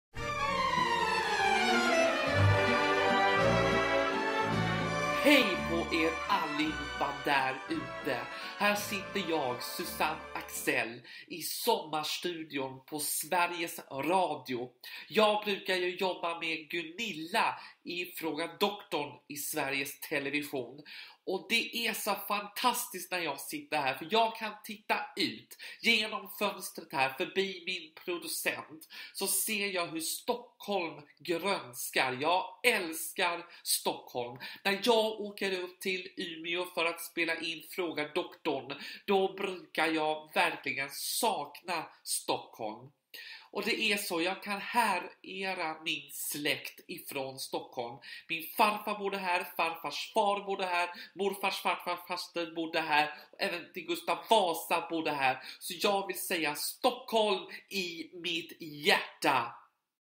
PARODI Sommar med Suzanne Axell
Dagens sommarpratare är "Suzanne Axell" (Magnus Carlsson), mest känd som programledare ”Fråga Doktorn” på SVT och hon pratar om sin hysteriska(?) kärlek till sin hemstad Stockholm.

Magnus Carlsson är artist, komiker och showproducent. Han medverkade i SVT's Gift vid första ögonkastet 2018.